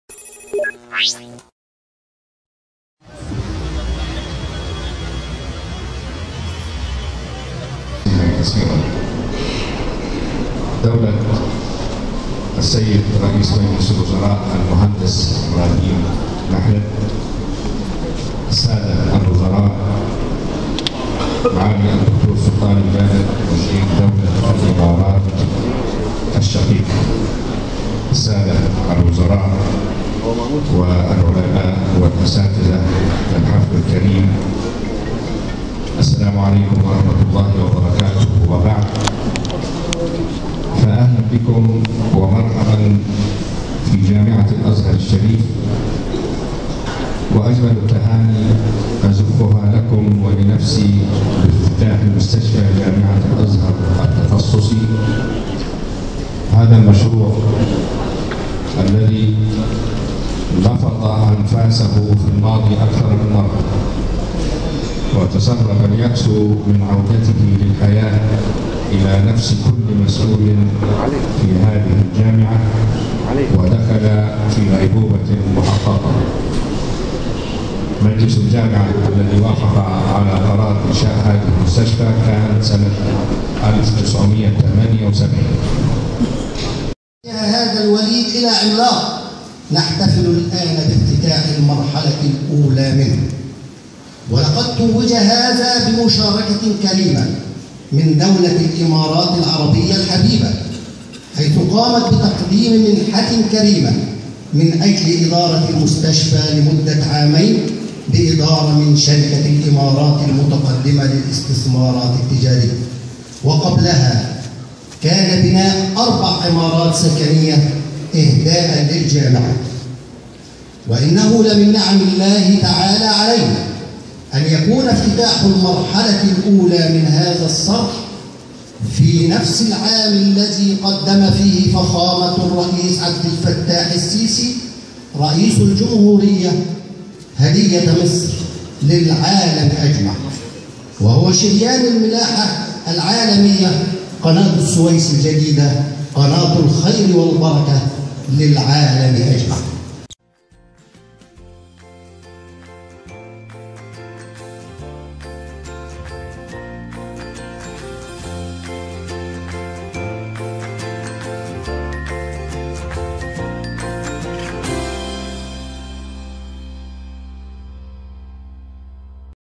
شفاف | افتتاح مستشفى الأزهر التخصصي بعد 37 عامًا من قرار إنشائها
افتتح اليوم، الأربعاء المهندس إبراهيم محلب رئيس مجلس الوزراء والأمام أحمد الطيب شيخ الأزهر الشريف، مستشفى الأزهر التخصصي   اليوم، الأربعاء، مستشفى الأزهر التخصصي بمدينة نصر، بعد 37 عامًا من قرار إنشائها.
تعد تلك المستشفى أحدث وأكبر مستشفى تعليمي في الشرق الأوسط، حيث تقام على مساحة 25 فدانًأ بتكلفة إجمالية اقتربت من المليار جنيه
• تقرير: نسمة نافع
• مونتاج : محمد الشافعي

•• شفاف .. أول شبكة إخبارية متخصصة في تغطية أخبار الجامعات ••